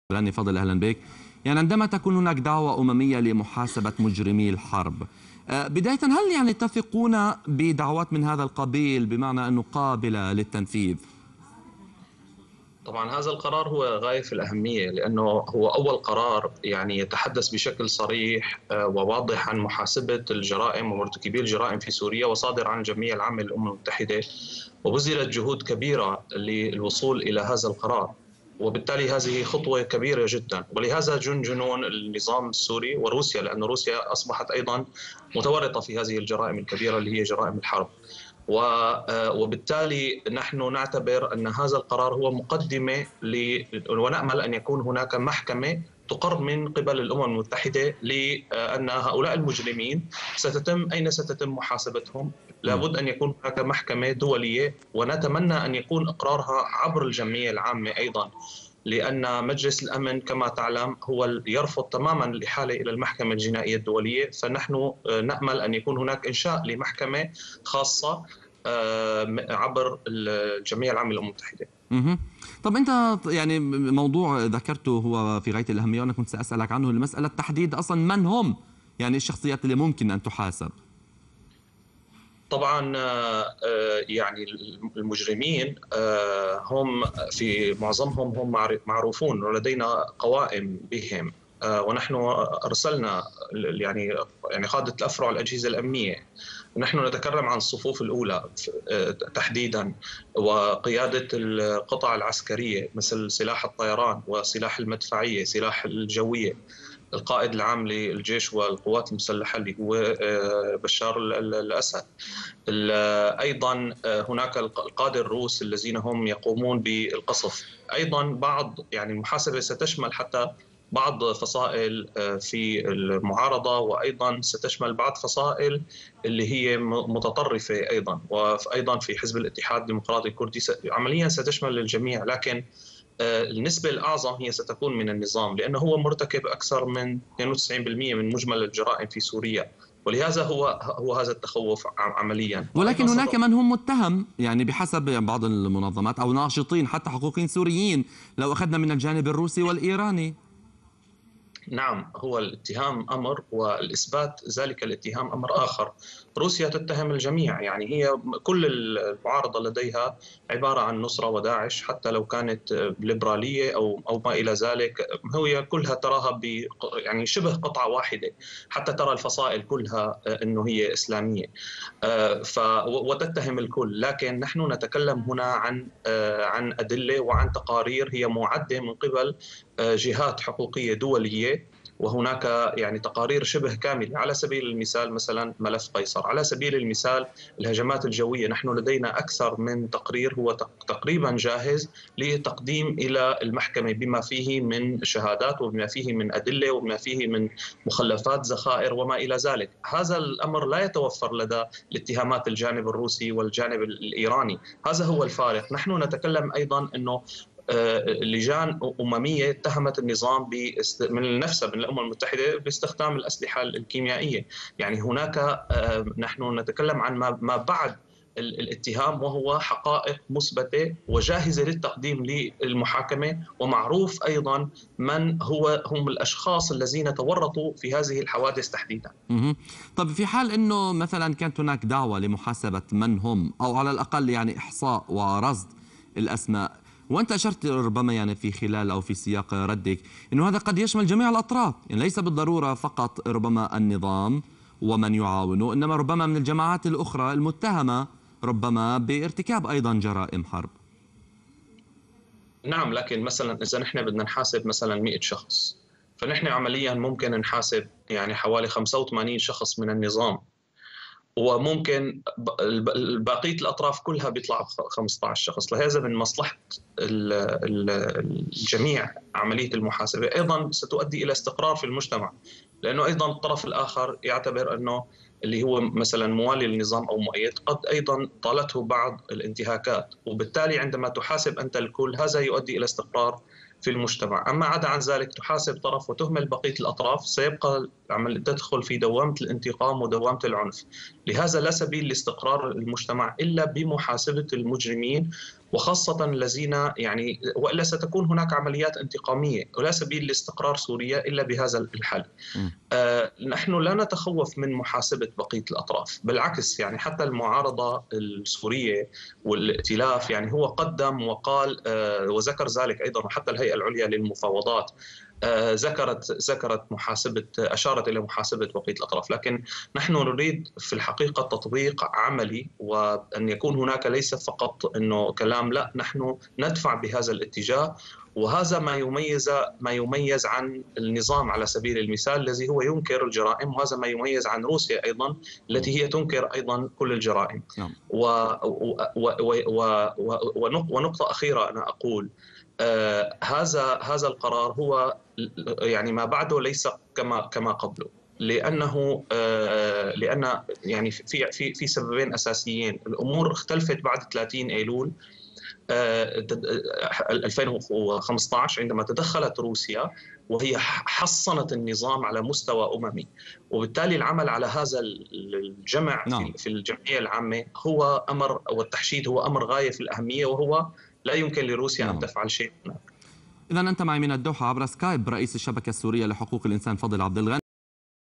Al Hadath Ch: Fadel Abdul Ghany on UN resolution for accountability in Syria 22\12\2016
تم اللقاء بتاريخ:
22/ كانون الأول /2016
قناة العربية الحدث: مداخلة فضل عبد الغني حول إقرار الأمم المتحدة مشروع لمحاسبة مجرمي الحرب في سوريا